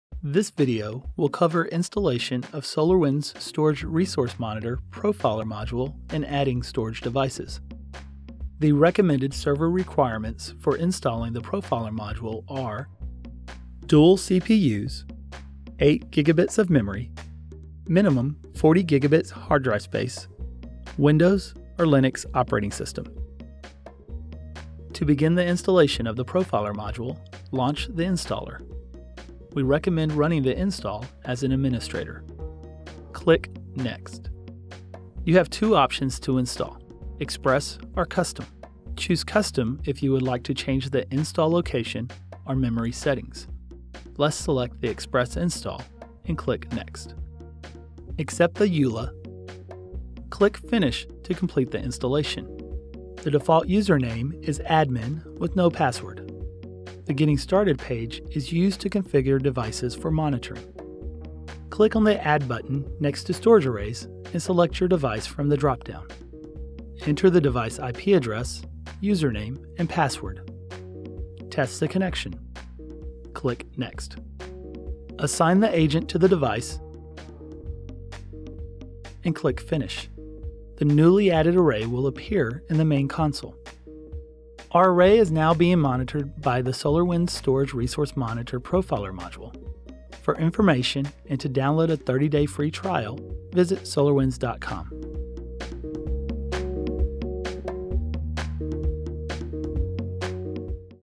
Getting Started with Storage Resource Monitor: Installing the Profiler Module
SolarWinds Storage Resource Monitor (SRM) makes it easy to monitor, diagnose and troubleshot storage performance and/or capacity issues in your multi-vendor storage environment. In this video we will walk through how to install Storage Resource Monitor, Profiler Module. After installation is completed, we will walk through the process of adding a storage device